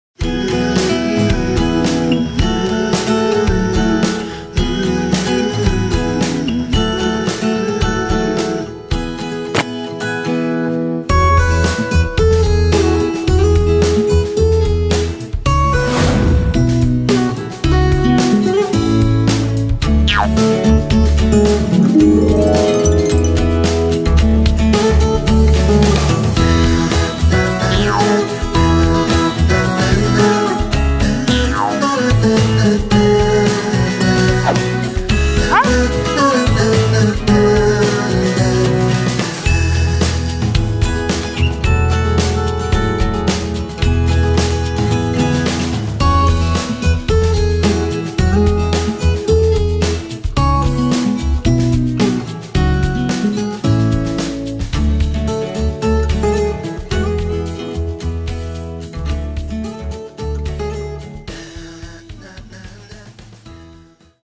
Make Elsa from Frozen PLAY DOH / Лепим Эльзу из Холодное сердце
Today you can see how quickly and easily you can sculpt from plasticine Elsa from Frozen ! PLAY DOH

Сегодня вы можете посмотреть, как легко и просто можно слепить Эльзу из Холодное сердце!